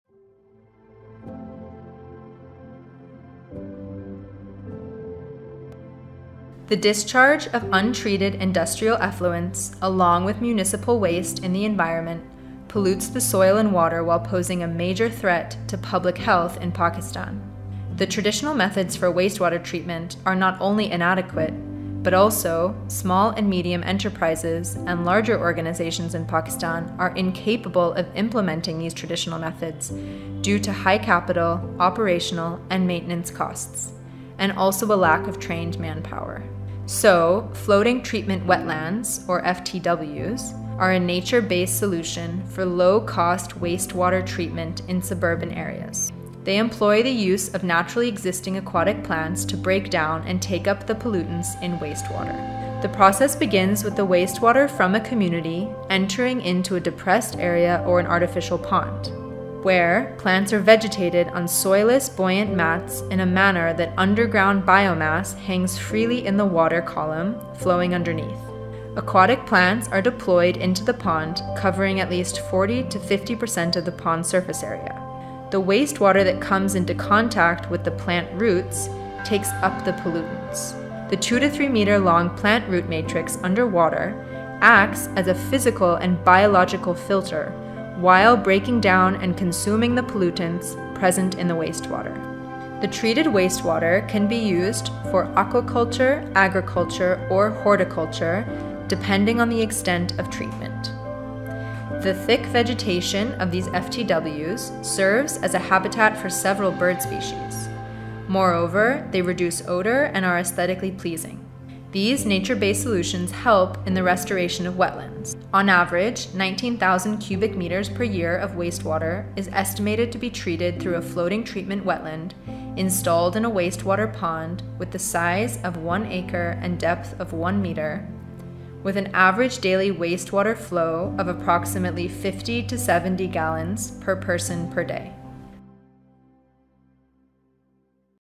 Floating Treatment Wetlands
Floating Treatment Wetlands (FTWs) are nature based solutions for suburban wastewater treatment.

DidYouKnow @wwfpak has deployed 11 FTW units across Pakistan? These interventions improve the quality of 136,600 m3 of wastewater each year.

Watch & join the wave of transformation! 🌊💧